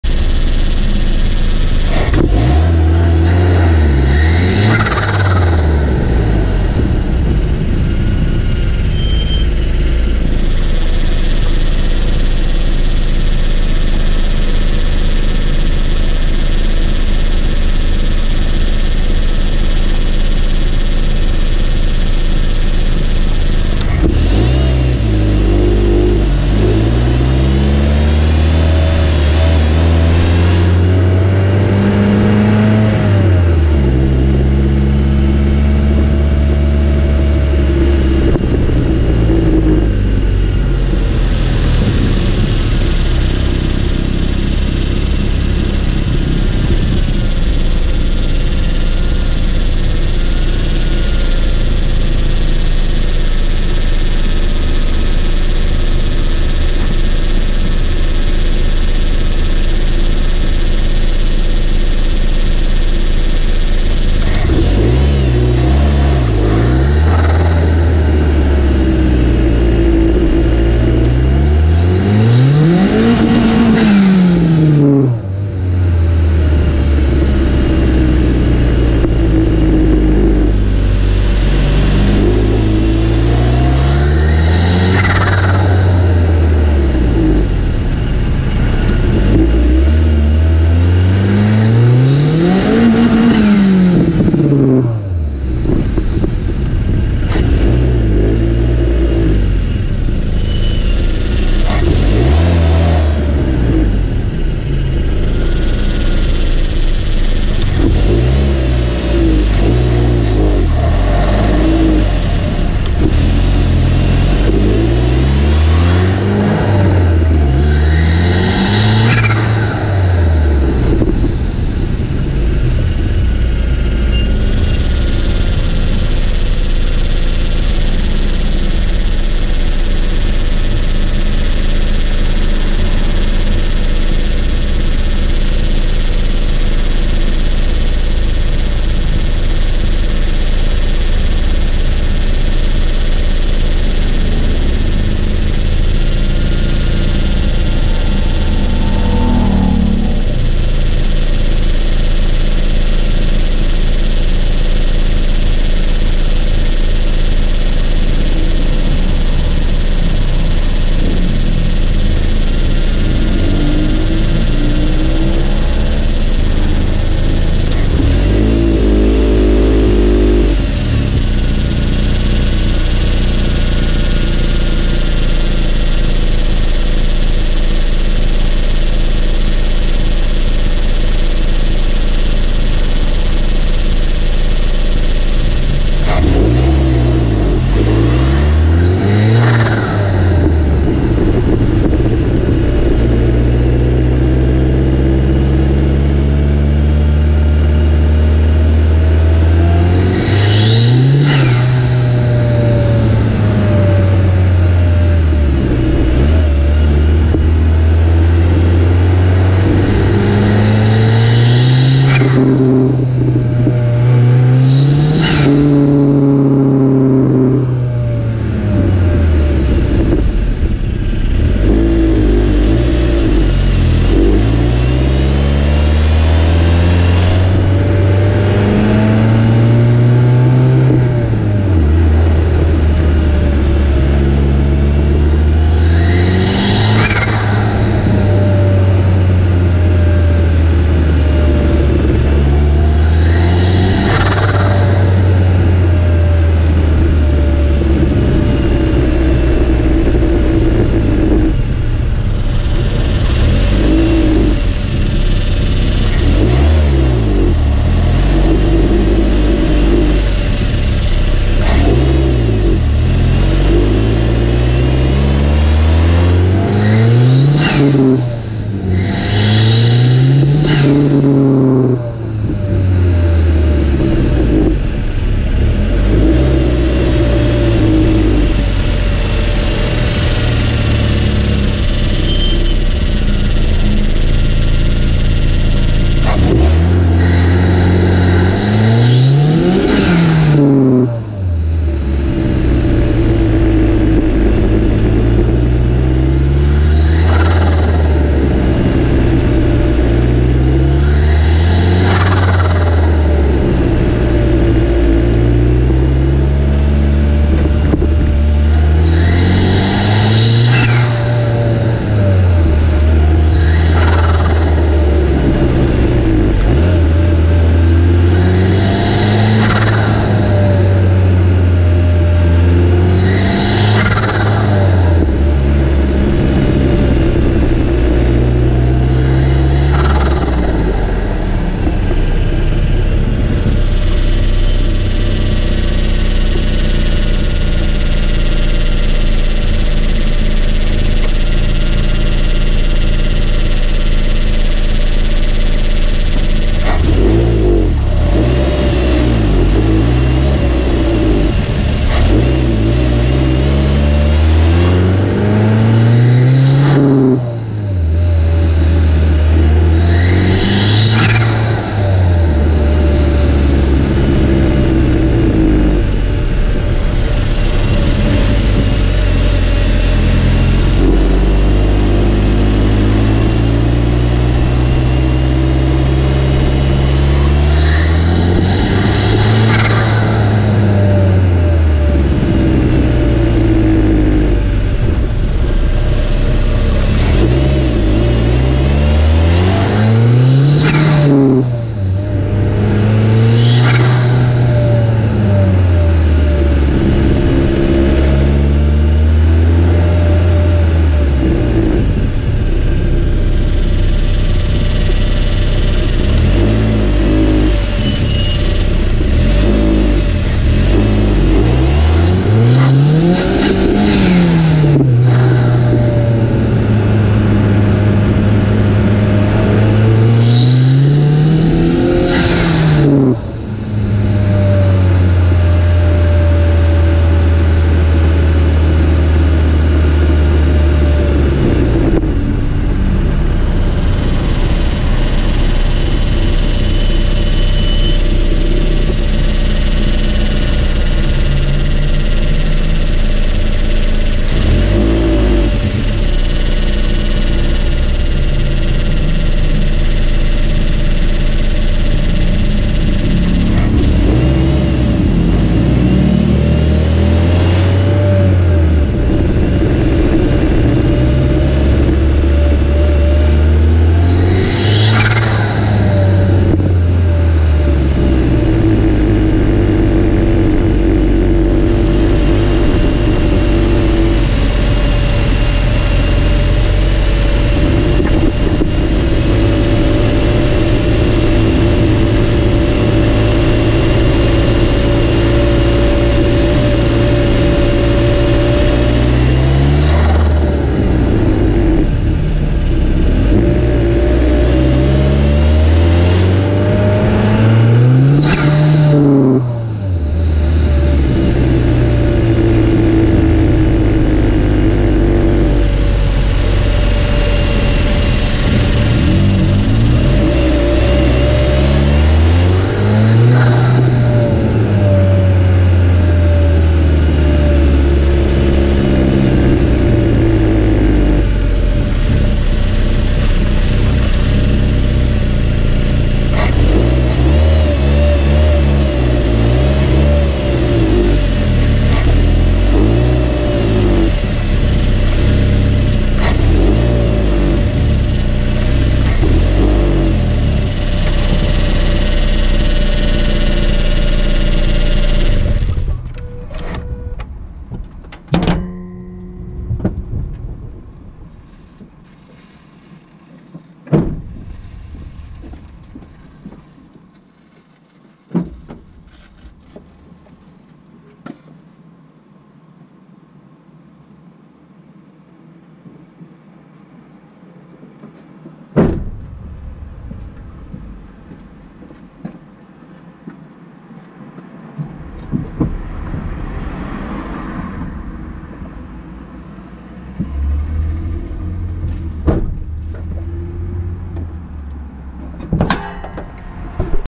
Mx5 NC 1.8 intake Sound Turbo
driving around town. testing a new gopro mount with led light